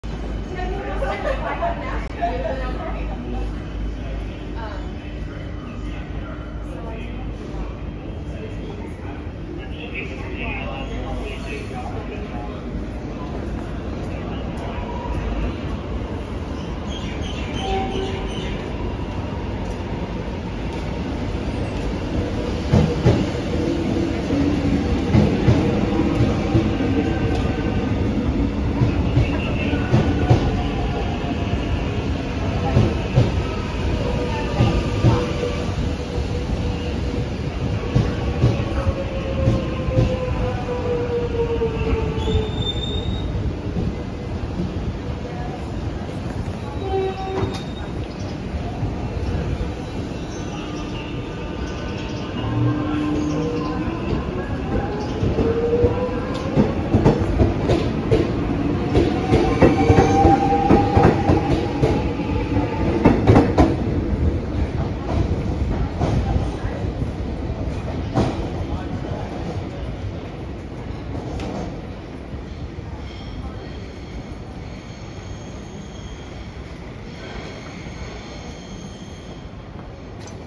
Metro Trains Melbourne [Lilydale] Alstom X'Trapolis 100 departing Flinders Street (Lilydale) 🇦🇺
Recorded on 20 September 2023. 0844hrs-0845hrs (MEL).

251M-1426T-252M & 915M-1676T-916M

Published on 21 November 2023, 1500hrs